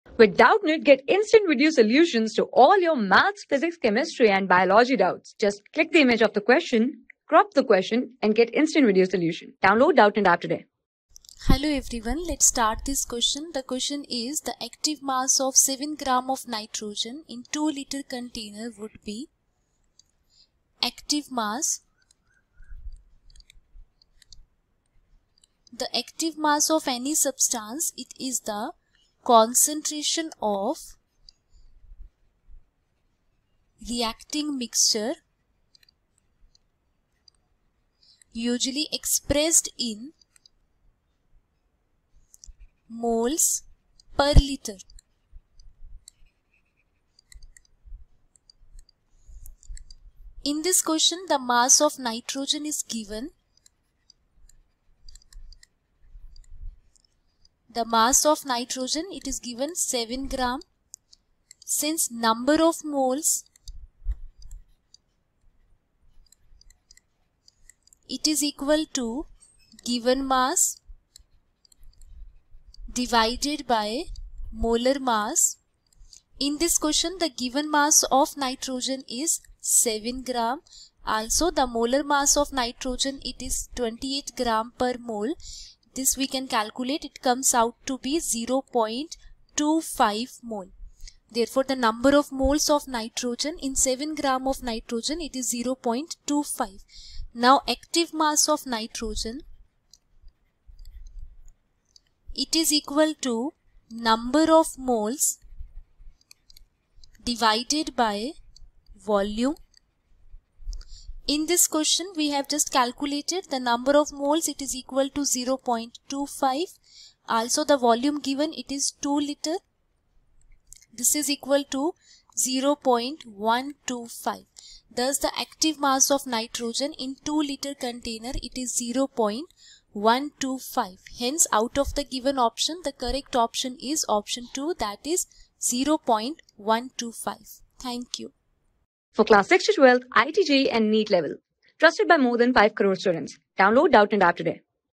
The active mass of 7.0 g of nitrogenin a 2.0 L container would be | 12 | EQUILIBRIUM | CHEMISTR...
The active mass of 7.0 g of nitrogenin a 2.0 L container would be
Class: 12
Subject: CHEMISTRY
Chapter: EQUILIBRIUM
Board:IIT JEE

👉 Have Any Query? Ask Us.